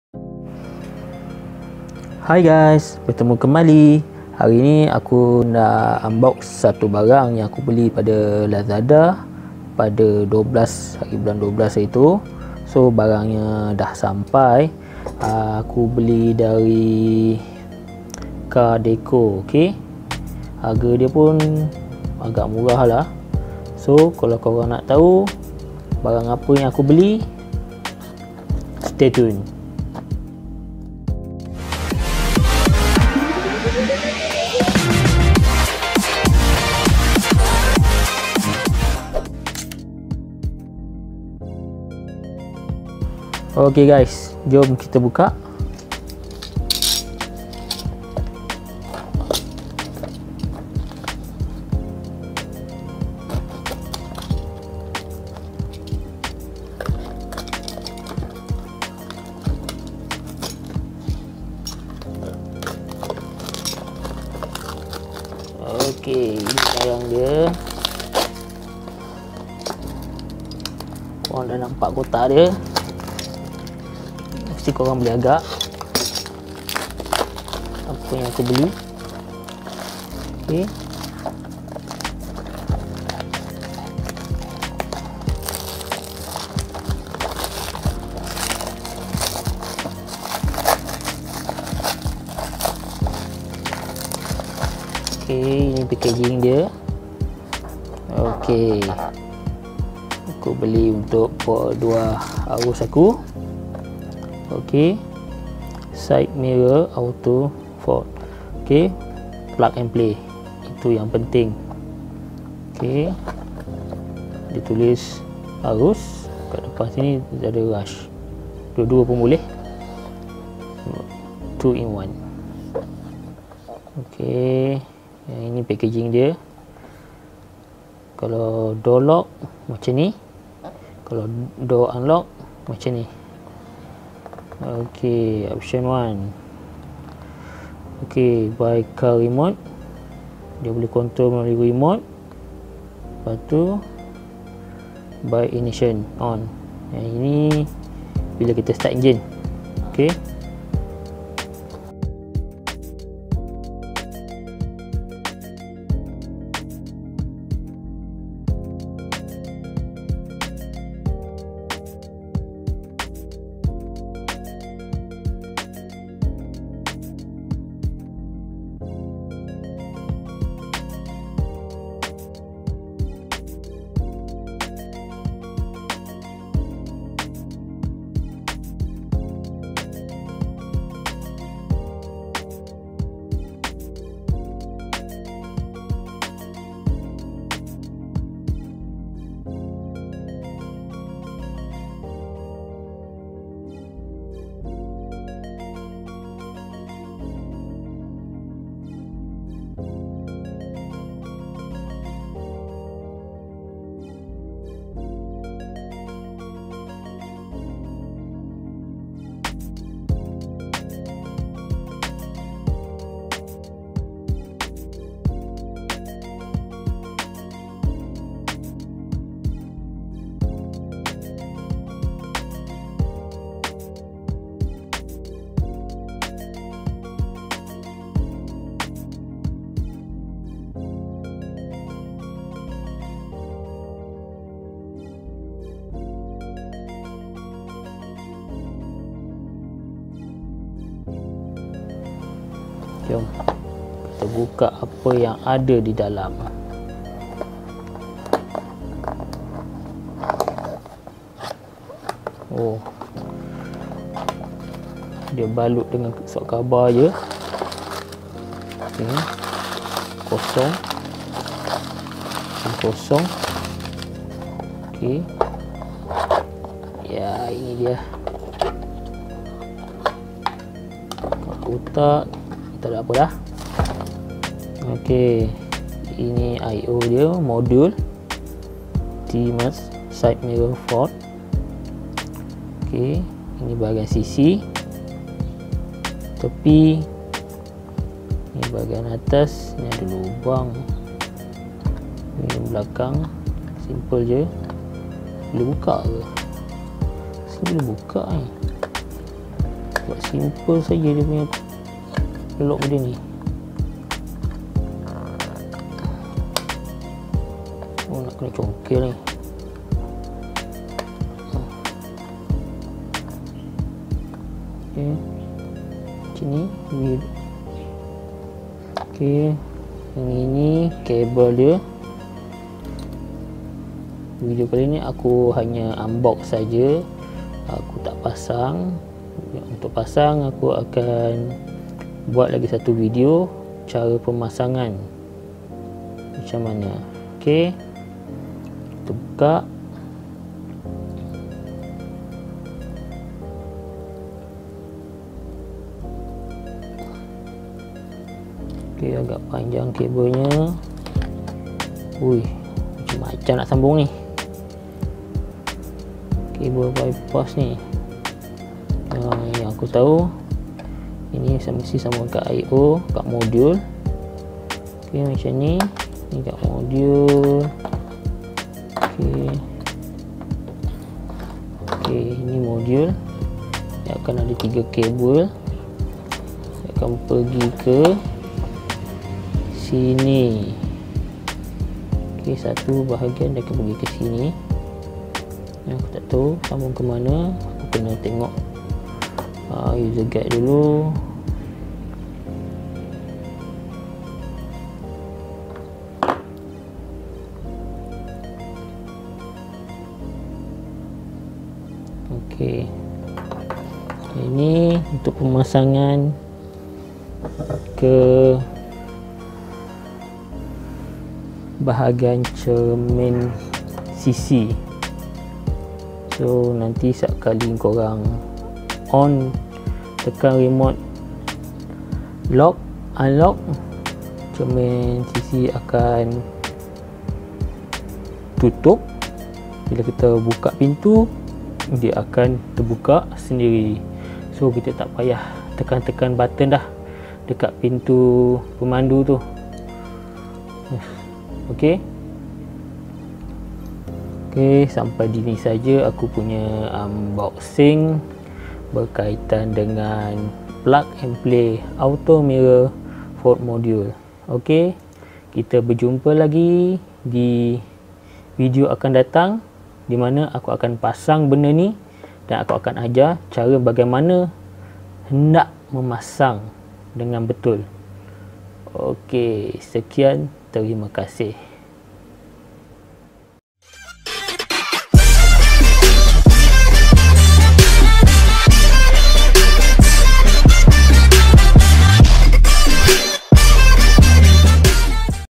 Unboxing Perodua Aruz Side Mirror Auto Fold
Didalam video ini, aku akan tunjukan hardware Side Mirror Auto Fold untuk Perodua Aruz